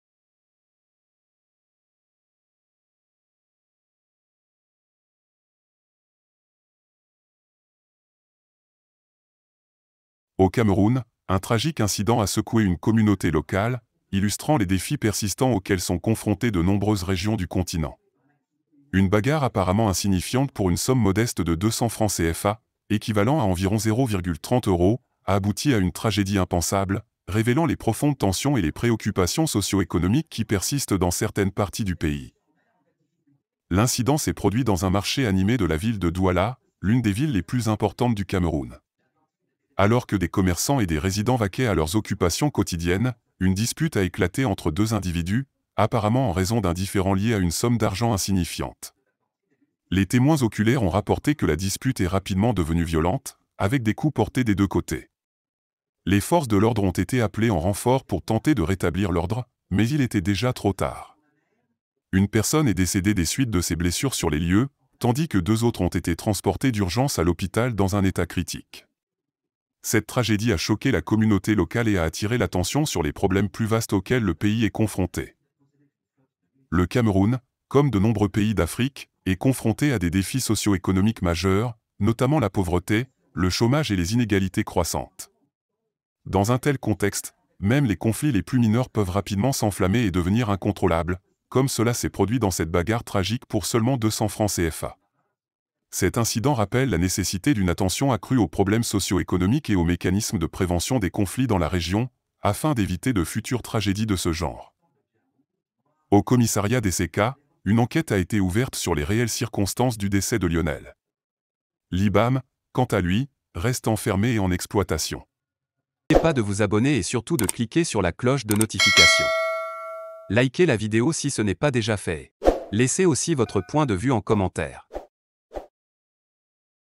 200 F CFA : Quand la Misère Déclenche la Violence au Cameroun
Au Cameroun, une bagarre pour la somme de 200 francs CFA, soit environ 0,30 euros, a dégénéré en une tragédie choquante dans un marché animé de Douala, l'une des principales villes du pays. Cette dispute insignifiante a abouti à un décès sur les lieux et à deux personnes gravement blessées. Cet incident souligne les profondes tensions socio-économiques qui prévalent dans certaines régions du Cameroun, mettant en lumière les défis persistants auxquels de nombreuses communautés locales sont confrontées. Ces tragédies rappellent l'urgence de s'attaquer aux problèmes de pauvreté, de chômage et d'inégalités croissantes pour prévenir de futurs conflits similaires.

La bagarre mortelle pour 200 francs CFA met en évidence les préoccupations socio-économiques au Cameroun, où des conflits mineurs peuvent rapidement s'enflammer dans un contexte de pauvreté, de chômage et d'inégalités. Cet incident tragique souligne la nécessité de s'attaquer à ces problèmes sous-jacents pour éviter de futures tragédies de ce genre. La communauté internationale doit prendre conscience de ces défis et soutenir des initiatives visant à améliorer la stabilité et la sécurité dans la région.